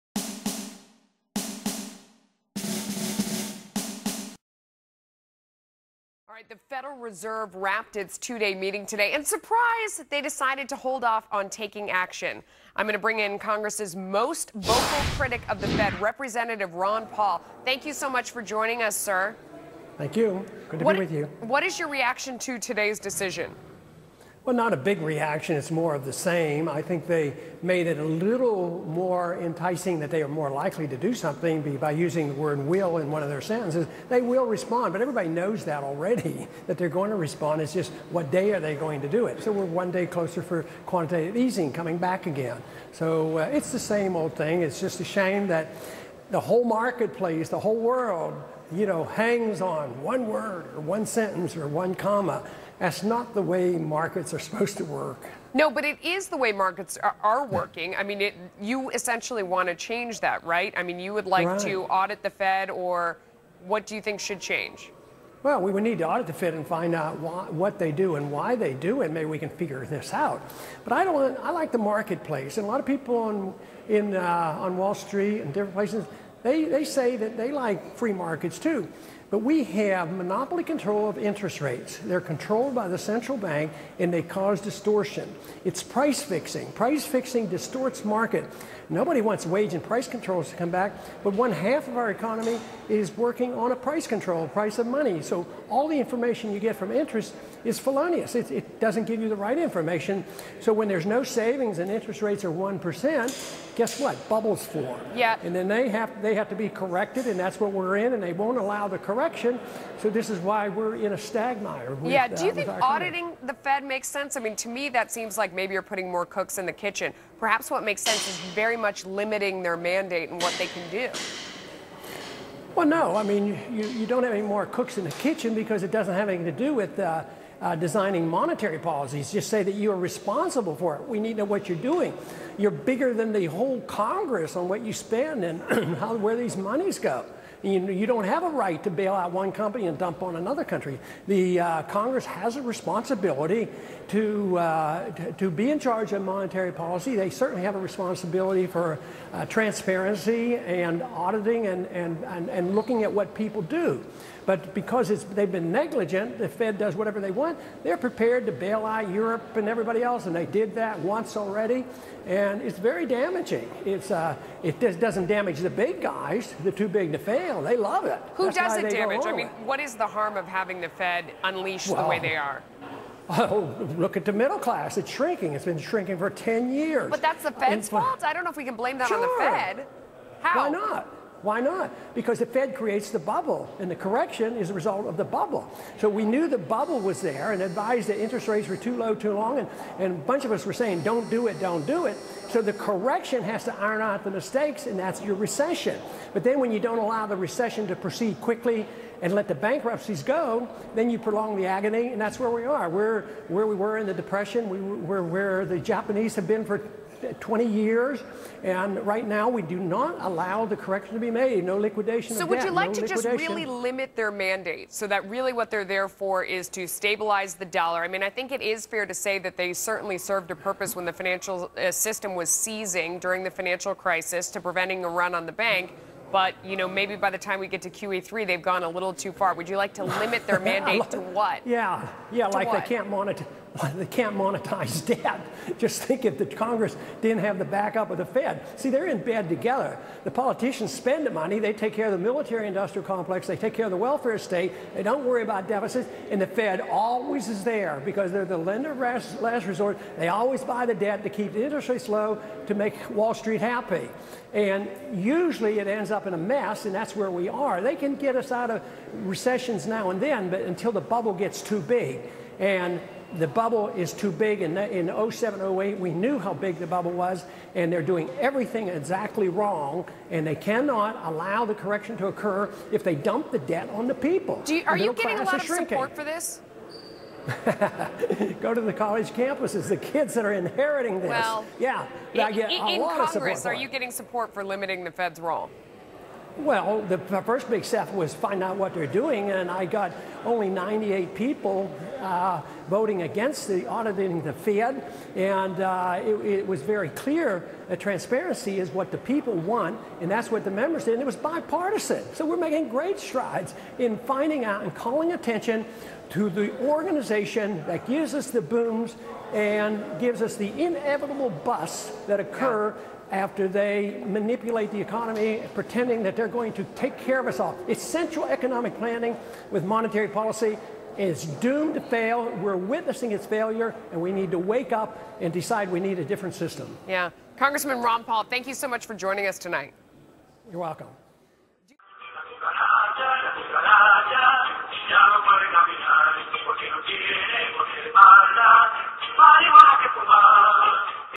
Ron Paul: Congress And Fed Are In Bed Together
Ron Paul on the Federal Reserve Monopoly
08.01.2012
Source: Fox Business News